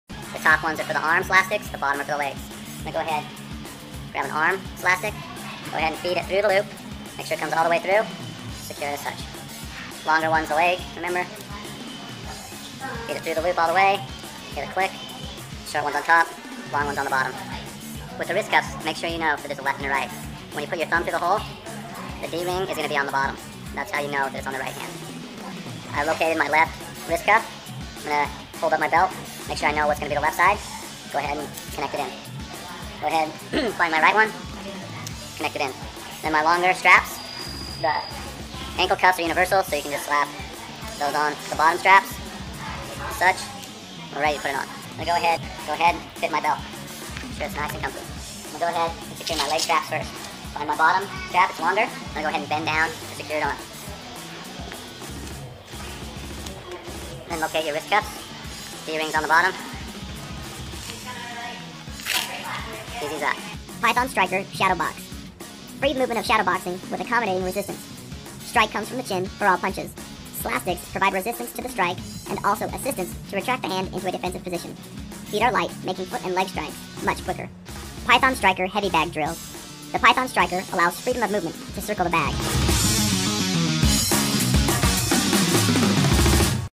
AMINEMARKET-WHAT IF YOU COULD TRAIN MORE EFFICIENTLY?
The Fight Band™ added resistance forces your muscles to work harder and grow strong. As a result, improving your general explosiveness, speed, and strength.